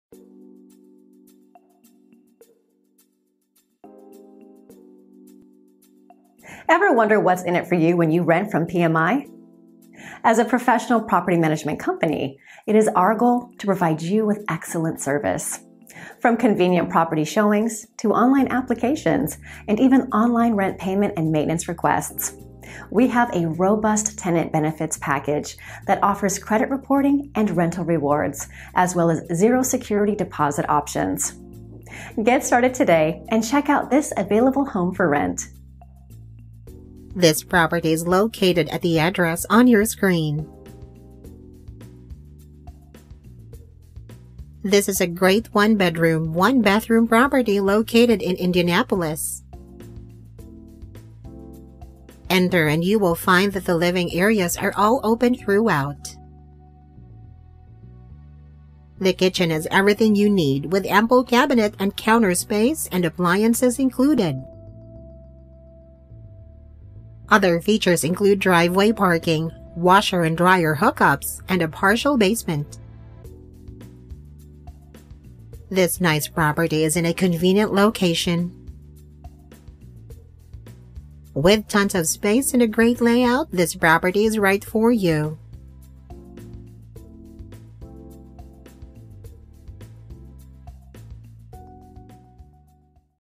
Property for Rent in Indianapolis 1BR/1BA by Indianapolis Property Managers
This is a great 1-bedroom, 1-bathroom property located in Indianapolis. Enter and you will find that the living areas are all open throughout. The kitchen has everything you need, with ample cabinet and counter space and appliances included. Other features include driveway parking, washer and dryer hookups, and a partial basement. This nice property is in a convenient location.

Looking for a property manager in Indianapolis? We do property management!

PMI Indianapolis
15275 Stony Creek Way suite a, Noblesville, IN 46060